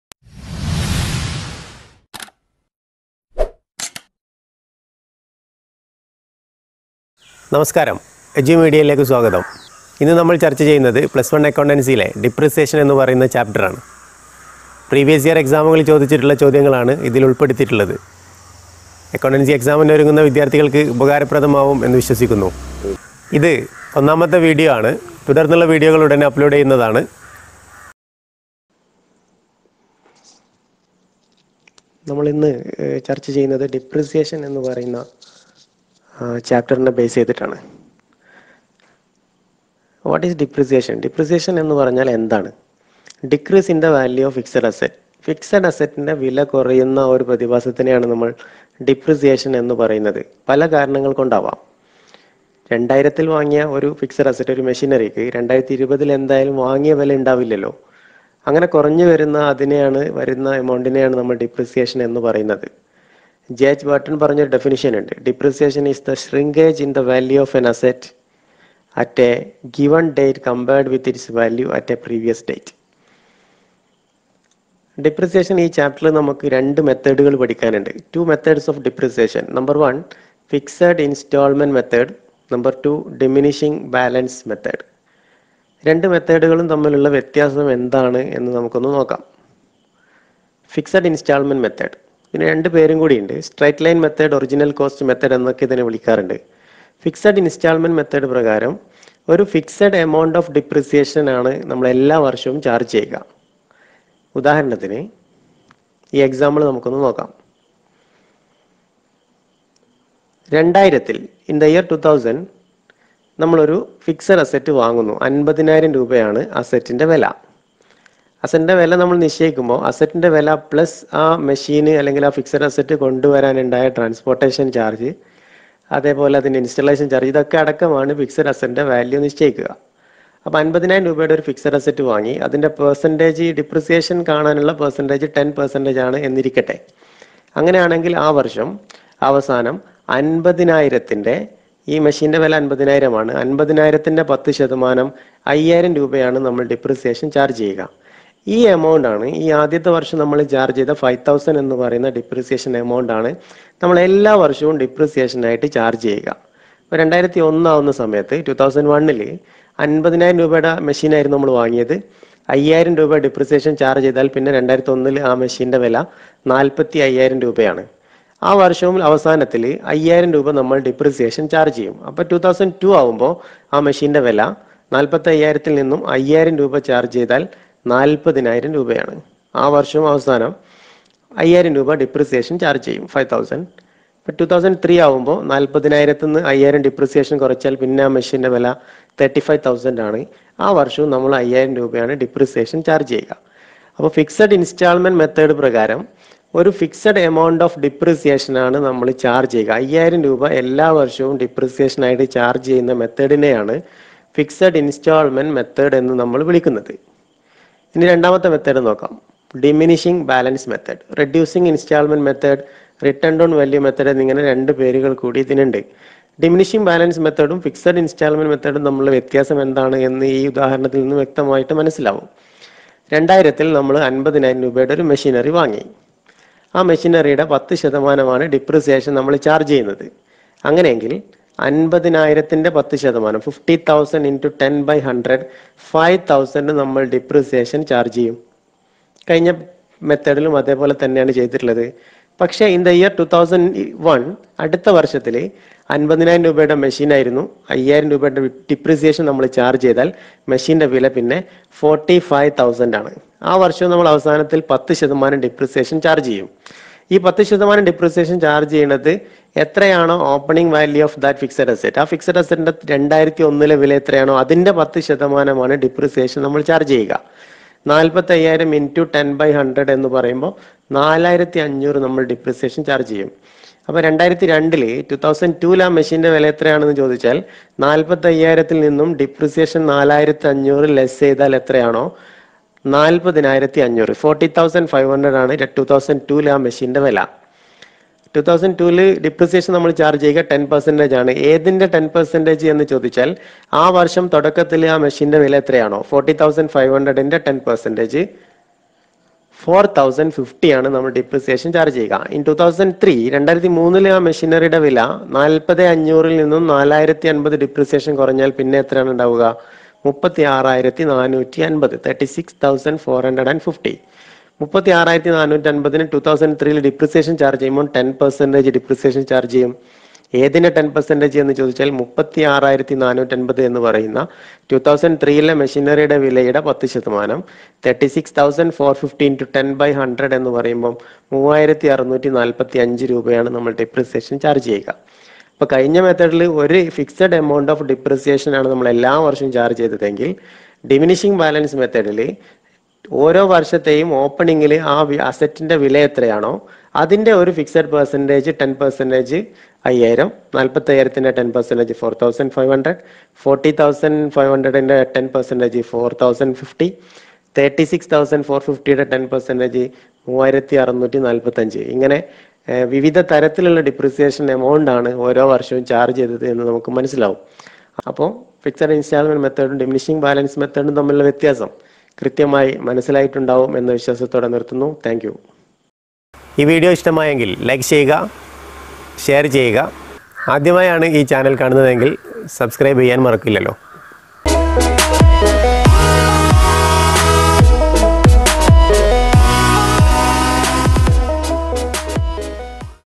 Plus One Accountancy Depreciation Part 1 - Malayalam
+1 Accountancy യിലെ Depreciation എന്ന ചാപ്റ്ററിലെ മുഴുവൻ മാർക്കും എളുപ്പത്തിൽ നേടിയെടുക്കാം